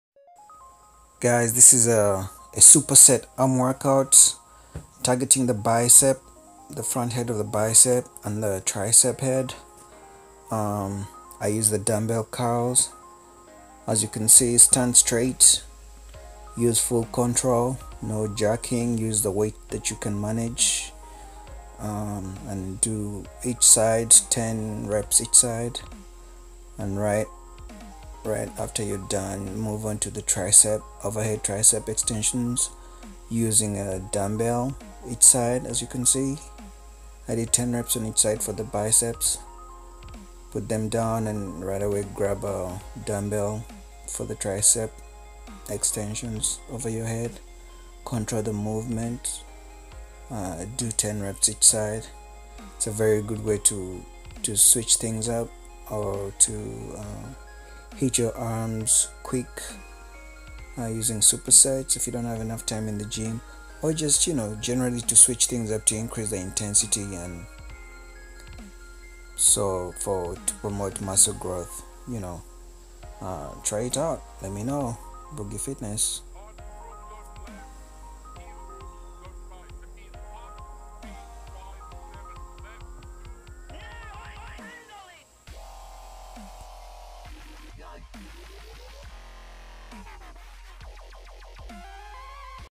BICEPS AND TRICEPS SUPERSET
Quick arms superset using dumbells for both Biceps and Triceps.